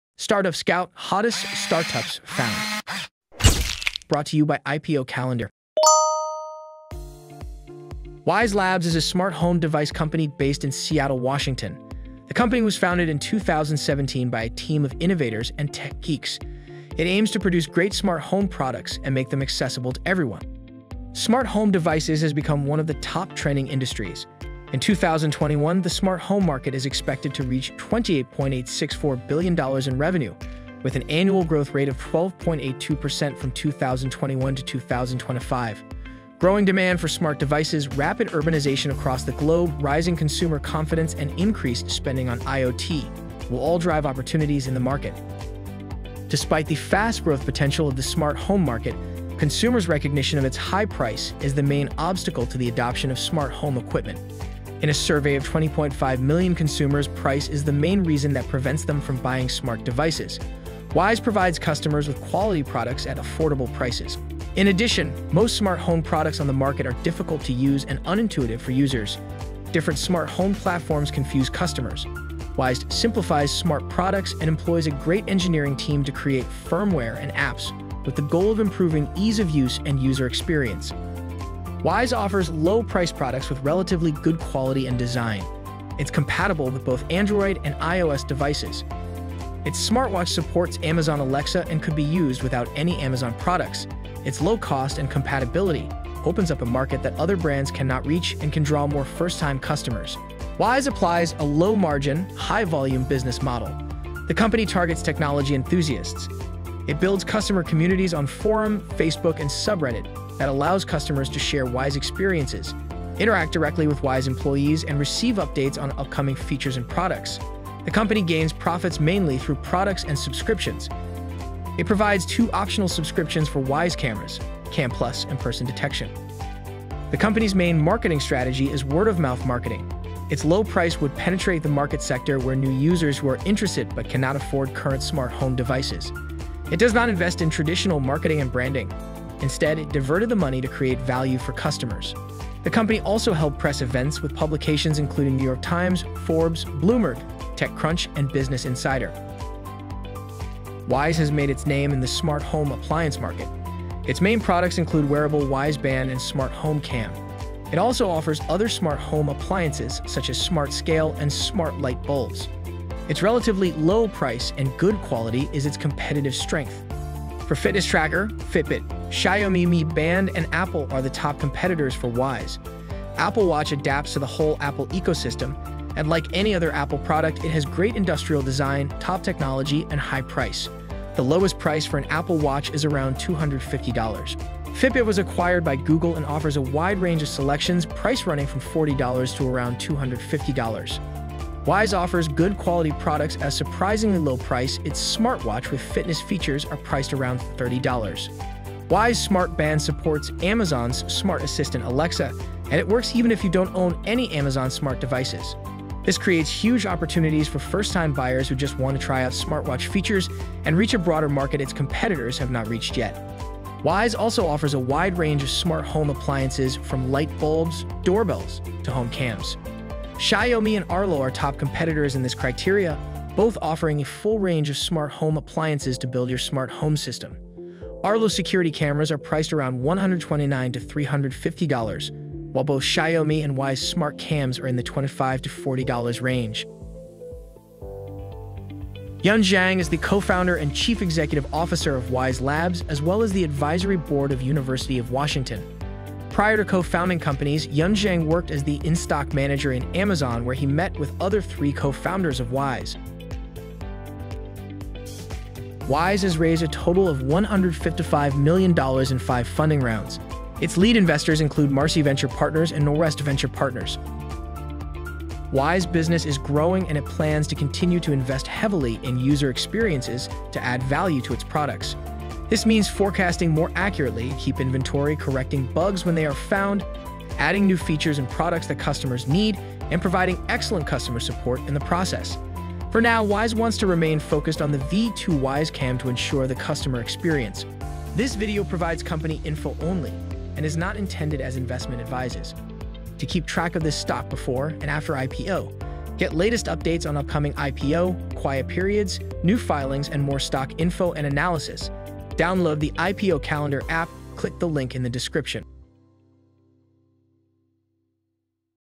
Wyze Labs | Startup Scout - Smart Home Device Company
Wyze Labs is a smart home device company founded in 2017 by a team of innovators and tech geeks. It aims to produce great smart home products and make them accessible to everyone. Here's an in-depth review of this company:
00:08 Startup Wyze Labs Intro
00:20 Wyze Opportunity
00:45 Wyze Value Proposition
01:21 Wyze Secret Sauce
01:40 Wyze Business Model
02:07 Wyze Go-To-Market Plan
02:29 Wyze Competitors: Apple, Fitbit, Xiaomi
03:57 Leadership
04:13 Wyze Financials
04:25 Wyze Future Plans